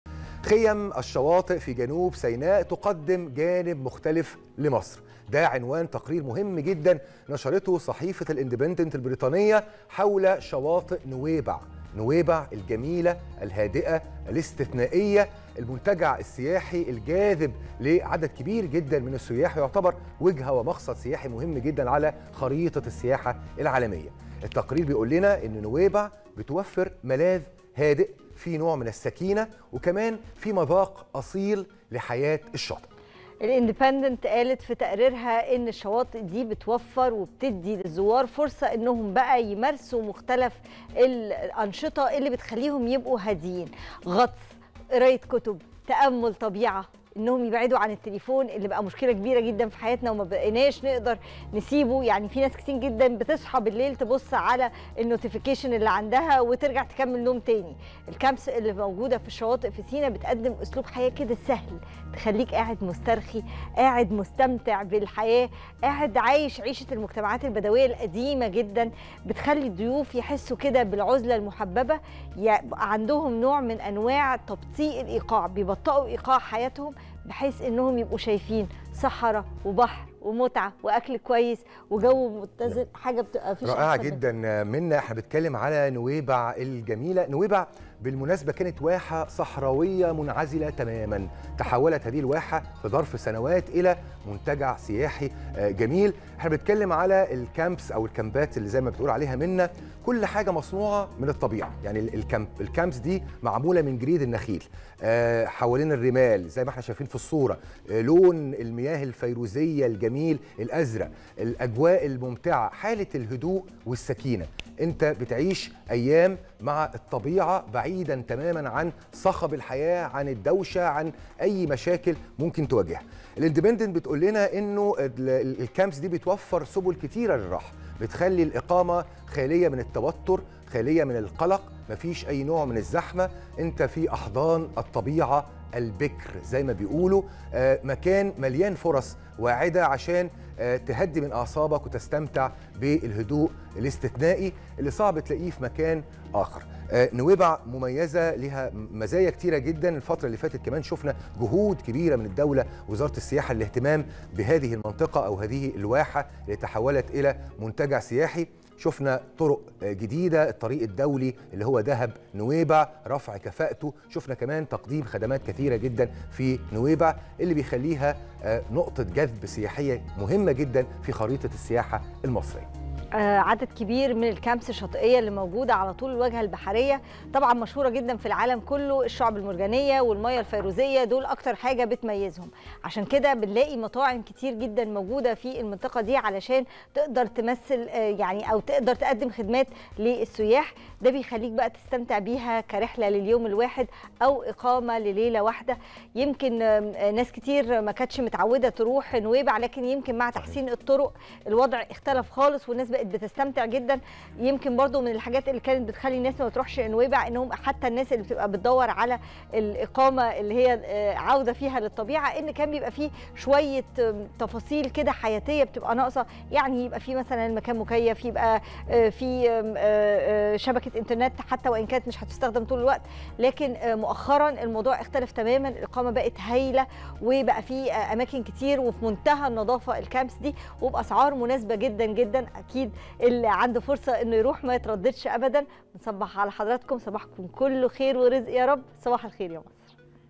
شواطئ نويبع الساحرة ملاذ هادئ ومذاق أصيل للمتعة بالشعاب المرجانية والمياه الفيروزية
صباح الخير يا مصر | شواطئ نويبع الساحرة ملاذ هادئ ومذاق أصيل للمتعة والواجهة البحرية الشهيرة بالشعاب المرجانية والمياه الفيروزية